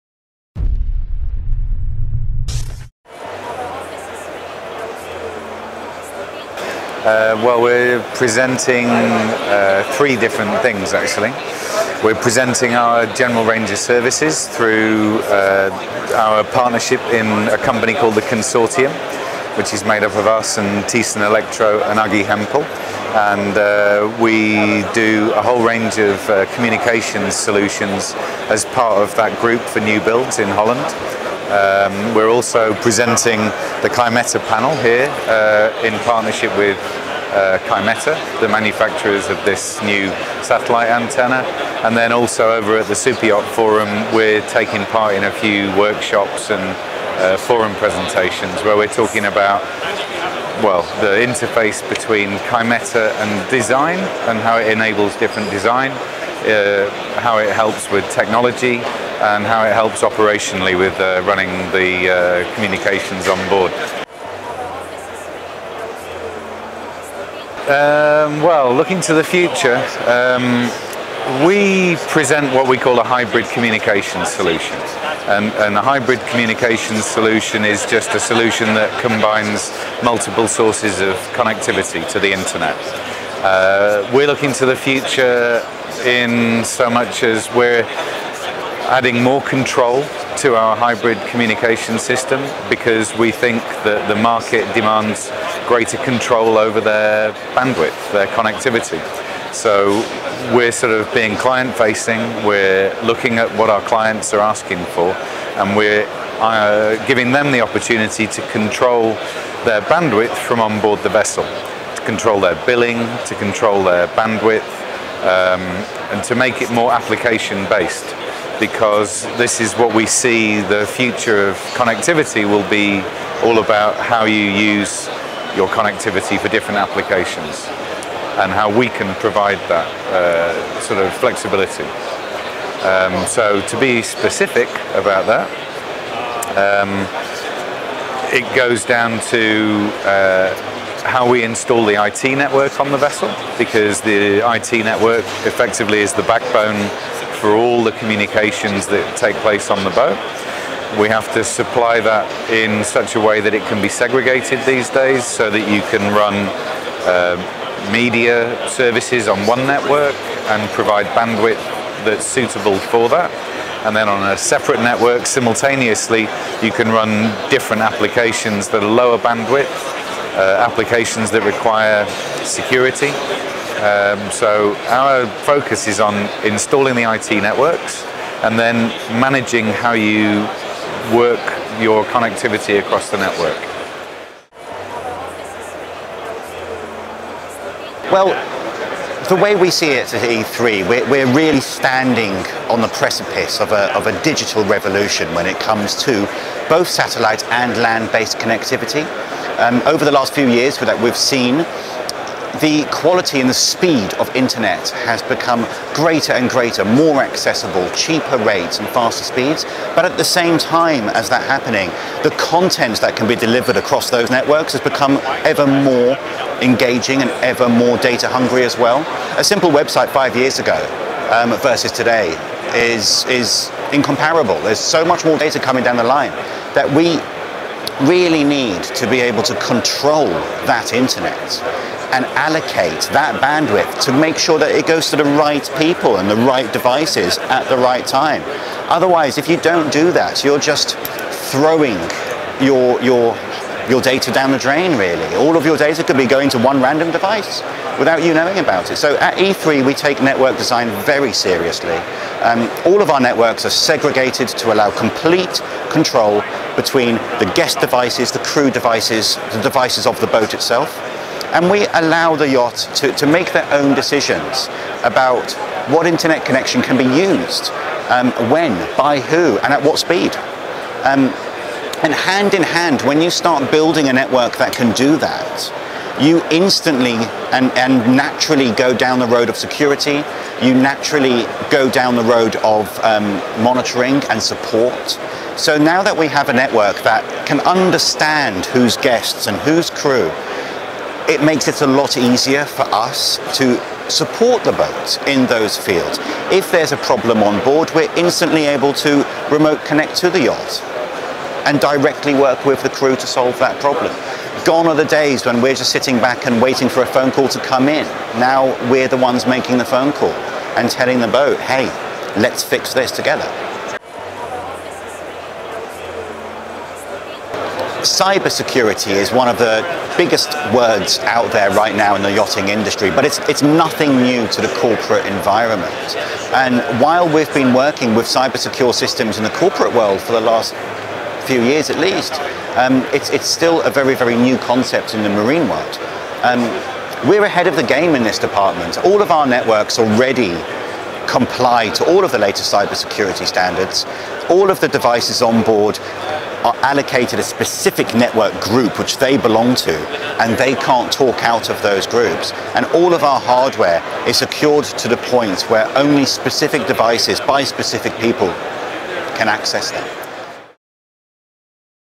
E3 - METS 2017
Which leading connectivity provider feels it is standing on the precipice of a digital revolution?

We hear from Greg Butler-Davis, European Sales Manager at e3 Systems, about market demand for greater bandwidth control. Adam Aquilina, the company’s Group IT Director, also fills us in on the modern connectivity challenges which run parallel to development.